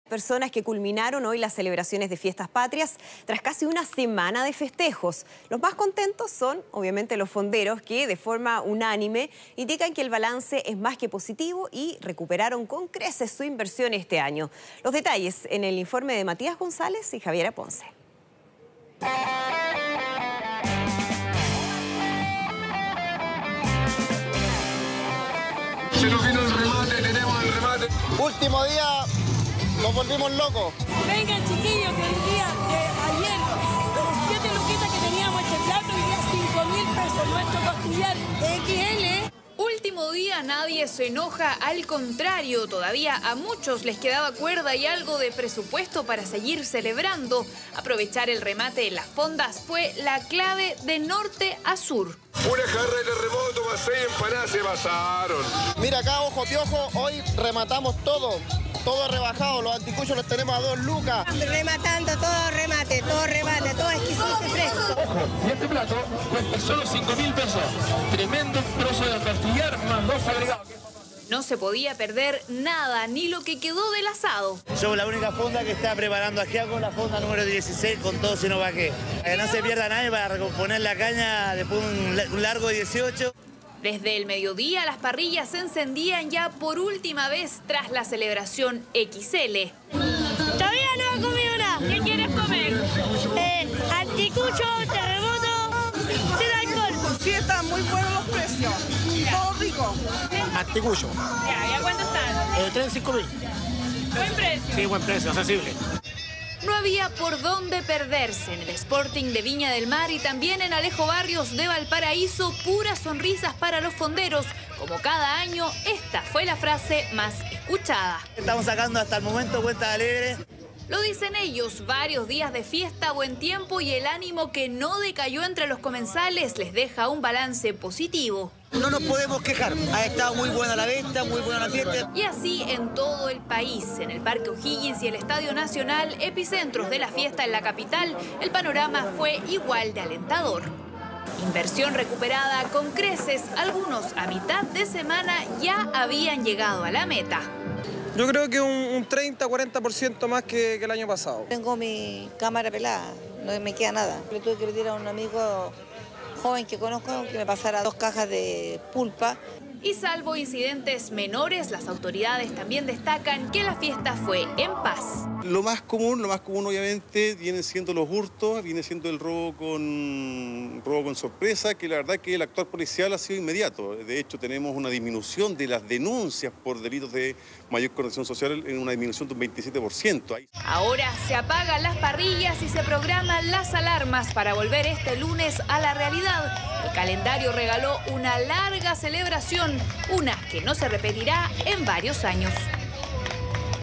Festejos dieciocheros: Fonderos remataron sus productos con balance positivo
Miles de personas a lo largo de todo el país culminaron las celebraciones de Fiestas Patrias tras casi una semana de festejos y los más contentos fueron los fonderos, que de forma unánime indican que el balance es más que positivo y recuperaron con creces su inversión este año.